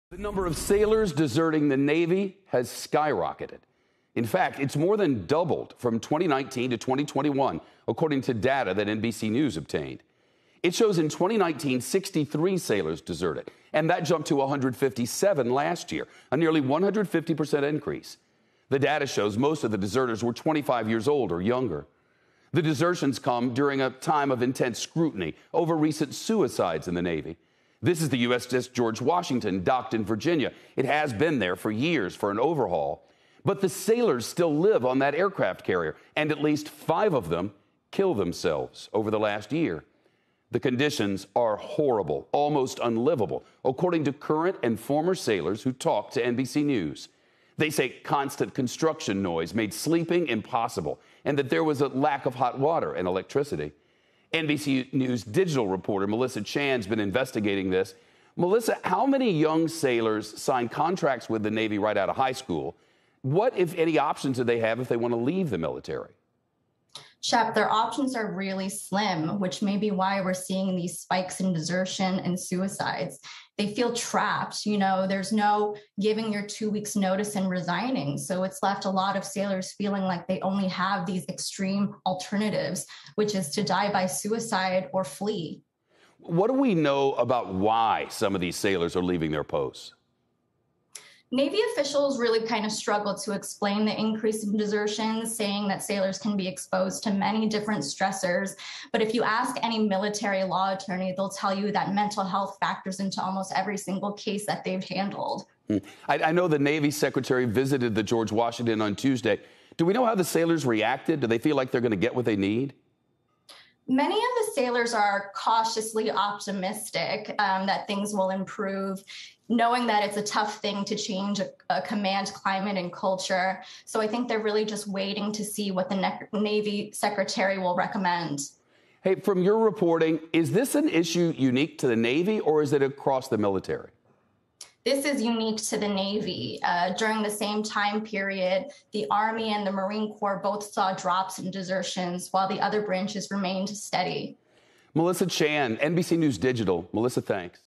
Desertions in the U.S. Navy skyrocket
NBC News Digital's Melissa Chan joins Shep Smith to report on desertion in the Navy, and why it's become such a big problem for that branch of the service. Sign up and learn more about the CNBC Investing Club with Jim Cramer